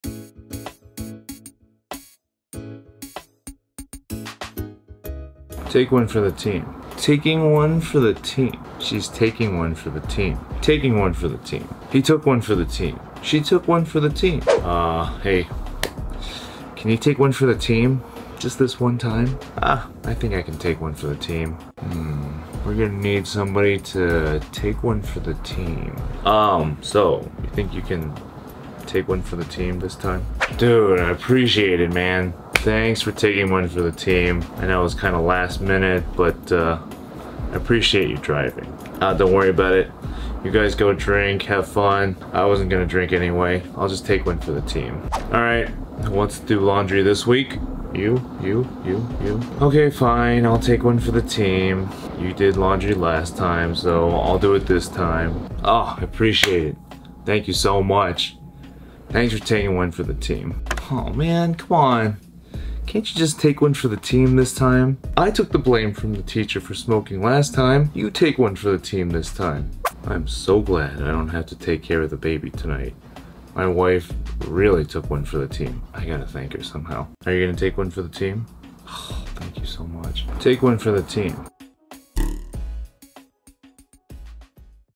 Take one for the team (What does it mean?)
Common ways to use the English phrase "Take one for the team"

Try to make your own example and write it in the comments!  Don't worry if it's not totally right.  I can help you with it.  It's fun to do!